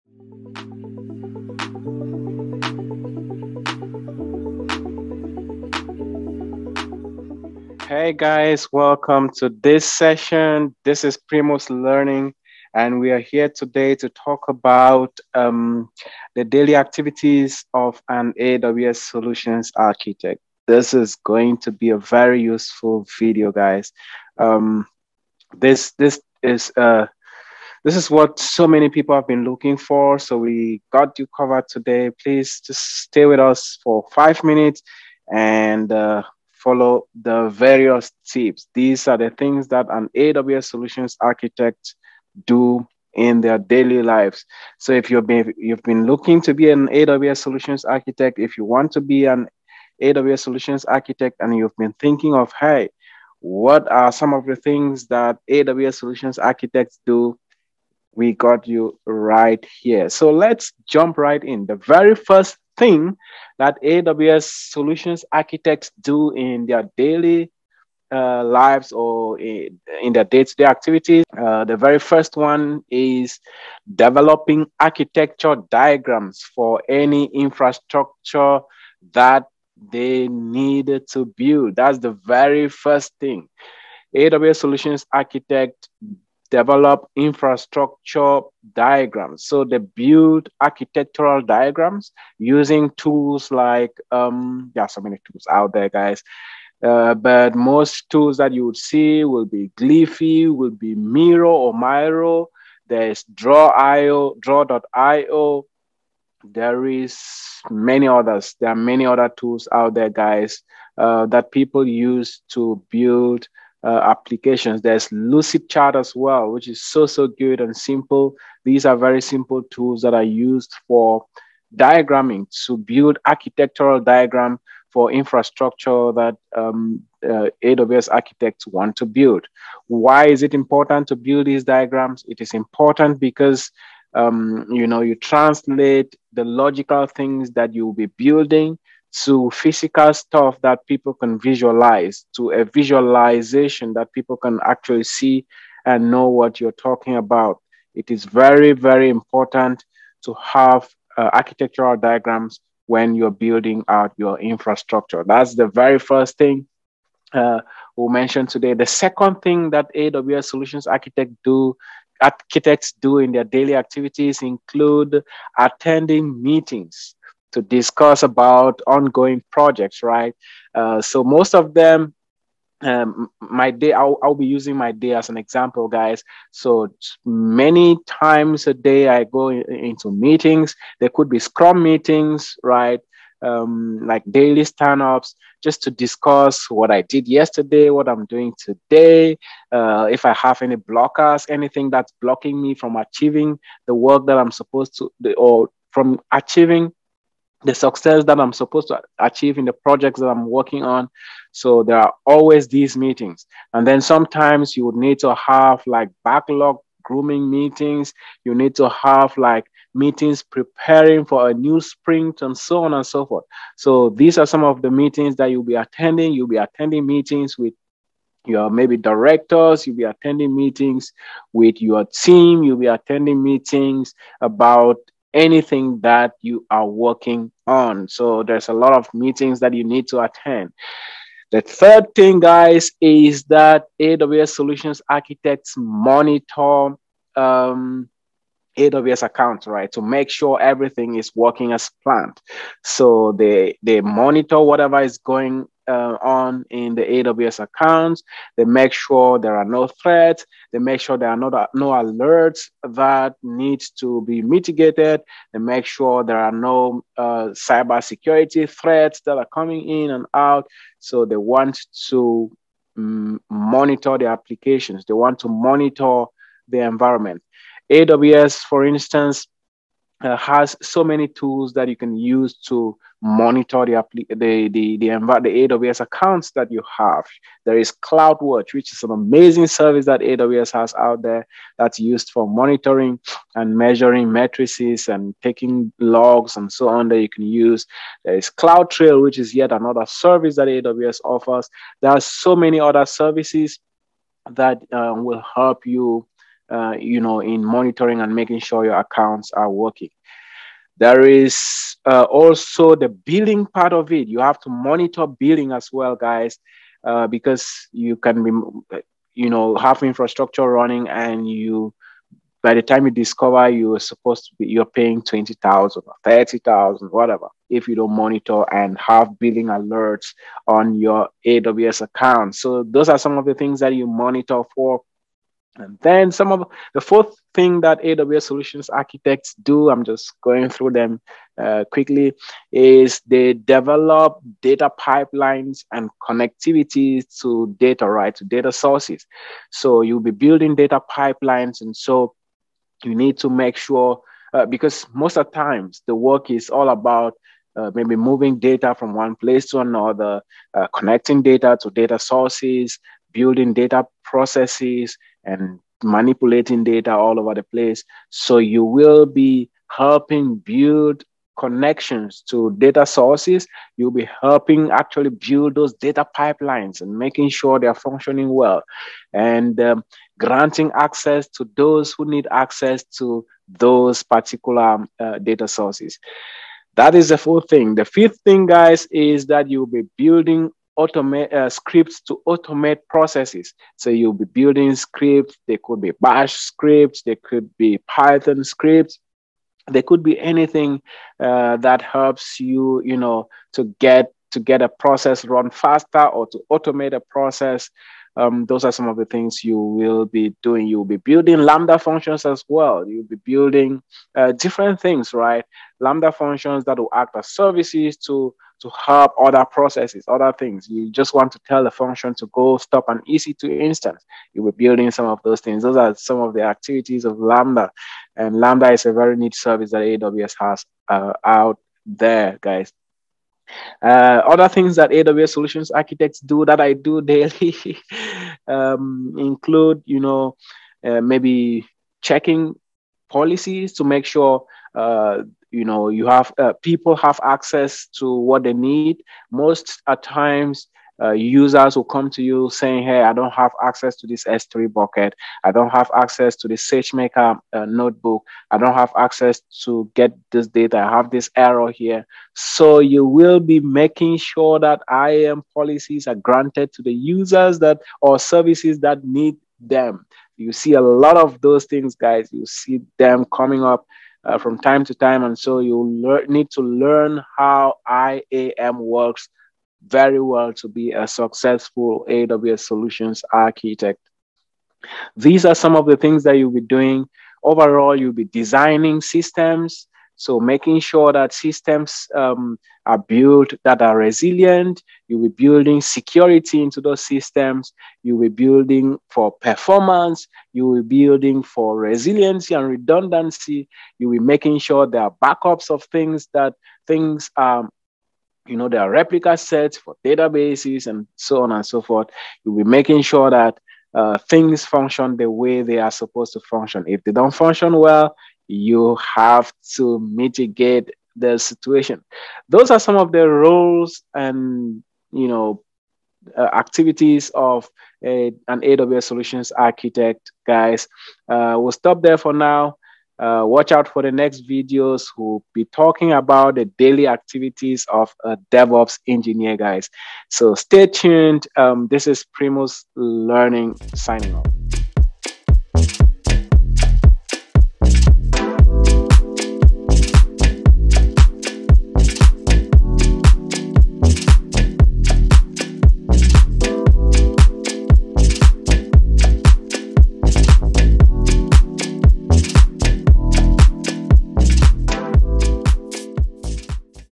Daily activities of an AWS Solutions Architect
Hey Primus Learning fam, today let's talk about the "Daily activities of an AWS Solutions Architect ". Let's watch and learn together.

Remember to let me know what I missed or what you have learned in the comment section.